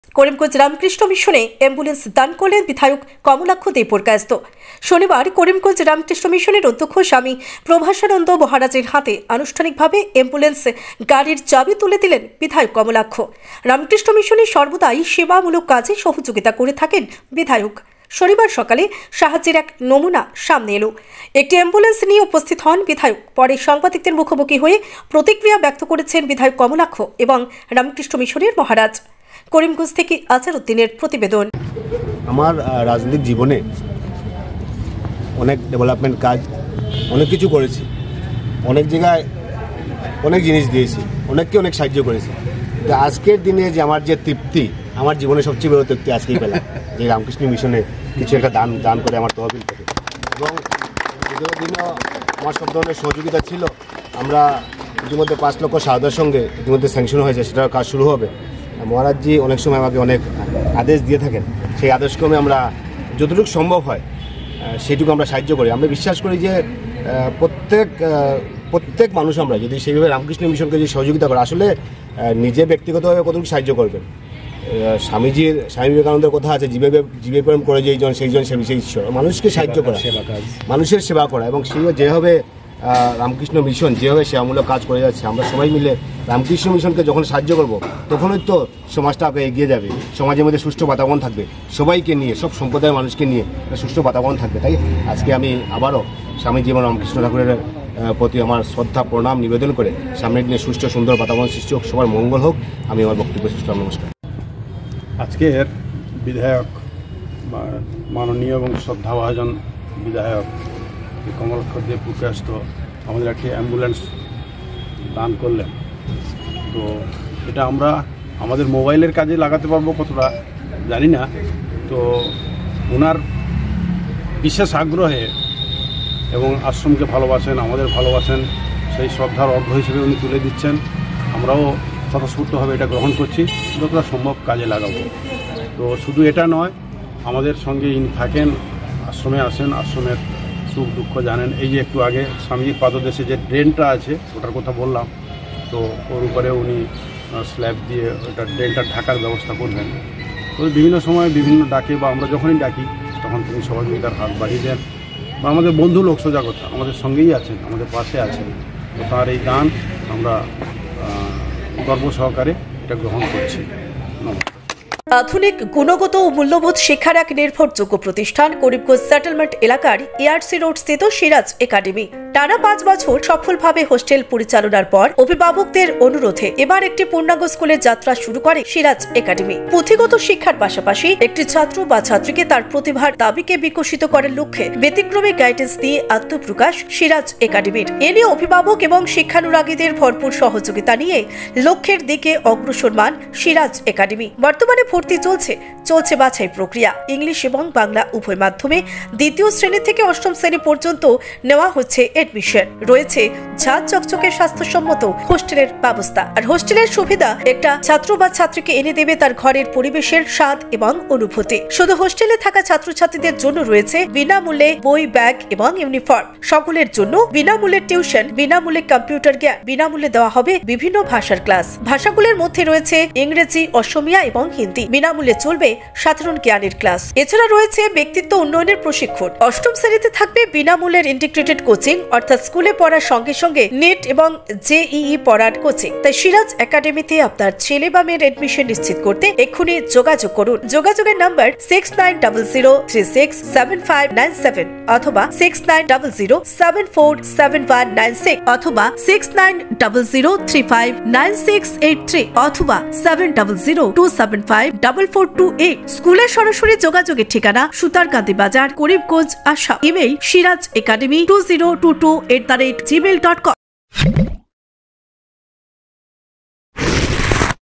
করিমগঞ্জ রামকৃষ্ণ মিশনে একটি অ্যাম্বুলেন্স দান করলেন বিধায়ক কমলাক্ষ
leanings.